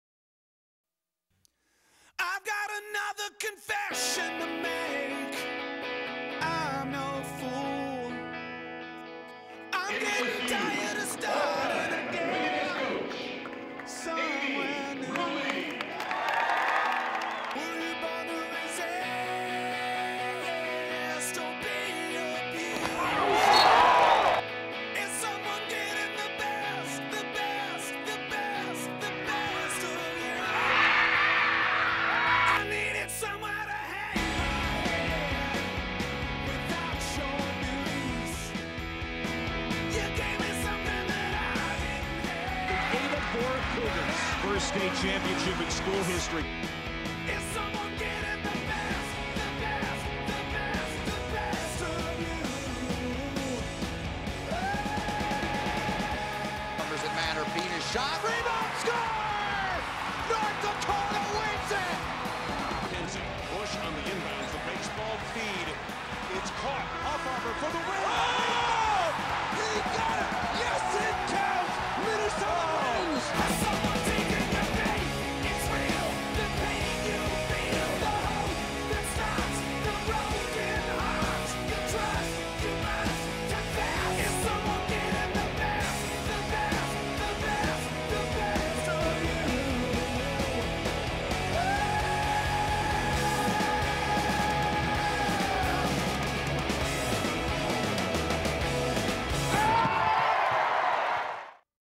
2008 Red River Valley Plays of the Year
A montage of the best highlights and moments from the 2008 sports year in the Red River Valley.